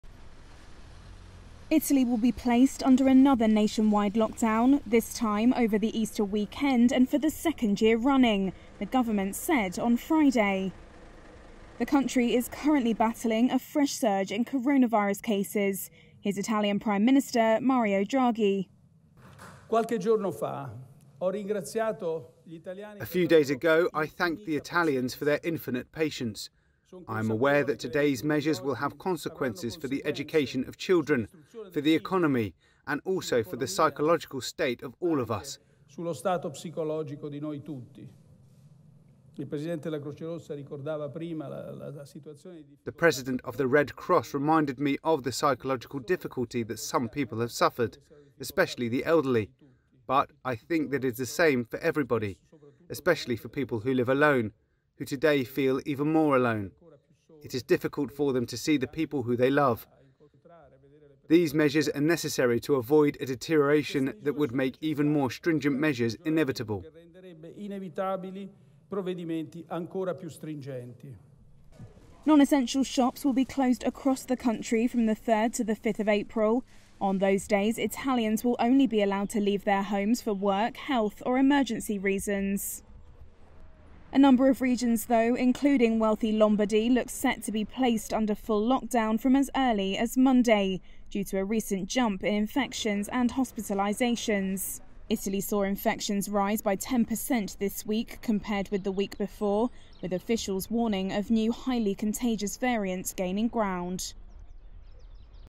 Italy to impose nationwide coronavirus lockdown over Easter weekend
Italy will be placed under a nationwide lockdown over the Easter weekend for the second year running, a draft decree law seen by Reuters said on Friday, underlining the struggle to stem a fresh surge in coronavirus cases.